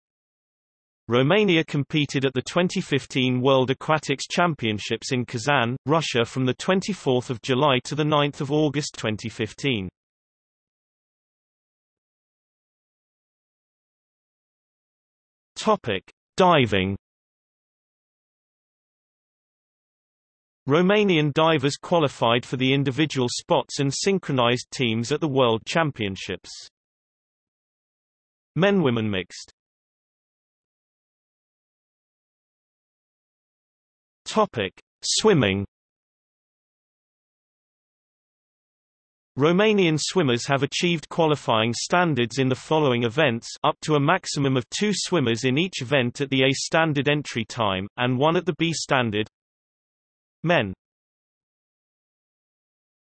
Romania at the 2015 World Aquatics Championships | Wikipedia audio article
00:00:11 1 Diving
00:00:30 2 Swimming

- Socrates

SUMMARY
Romania competed at the 2015 World Aquatics Championships in Kazan, Russia from 24 July to 9 August 2015.